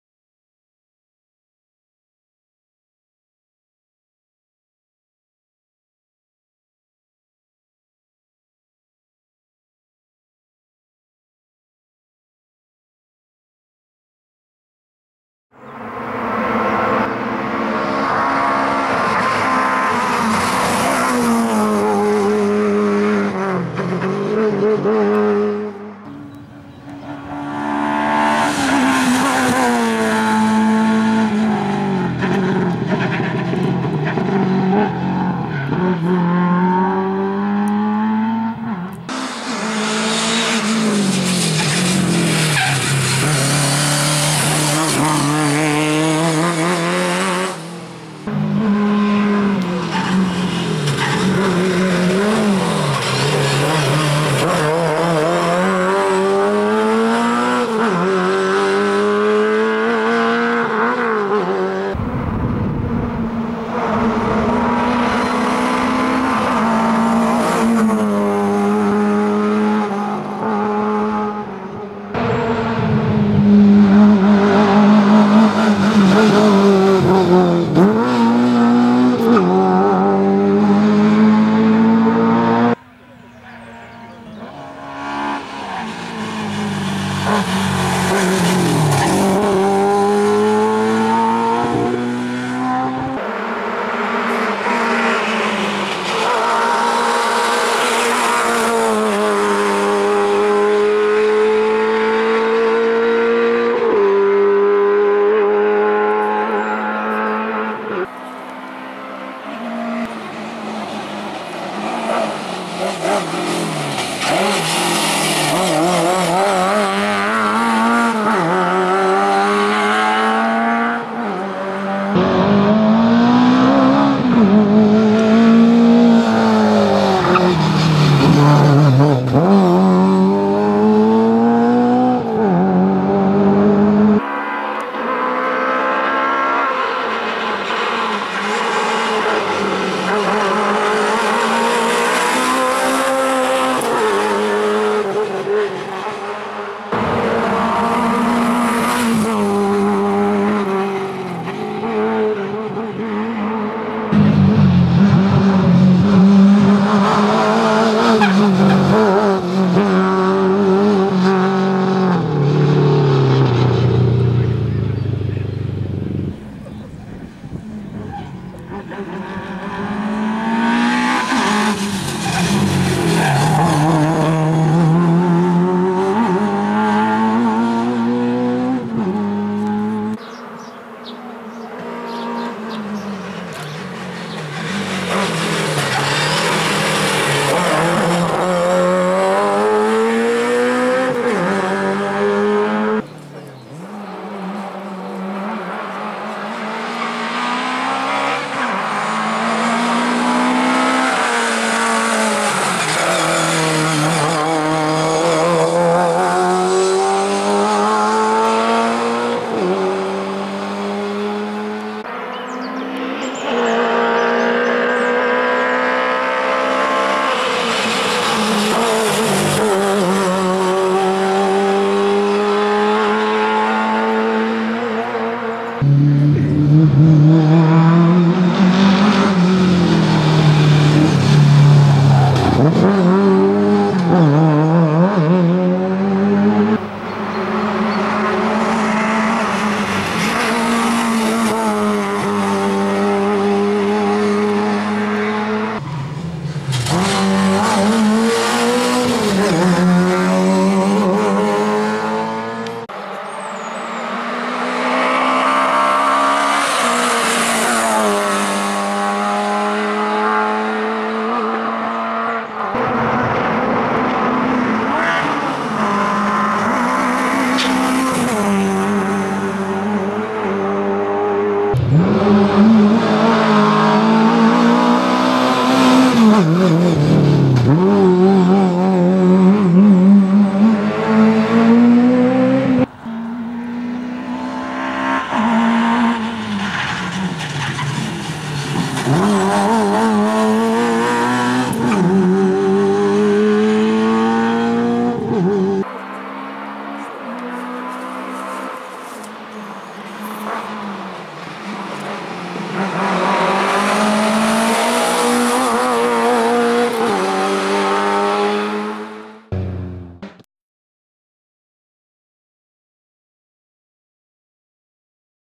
ifj.Vojtek-Molnár Lada 2107 A123 Teszt Rallye 2016.-Lepold Sportvideo
ifj.Vojtek Gábor és Monár Zsolt versenyzői filmje,a 2016-os orfűi A123 Teszt Rallye-ról.Vágó:Dénes Szabolcs Szerkesztő:Lepold Zsolt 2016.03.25.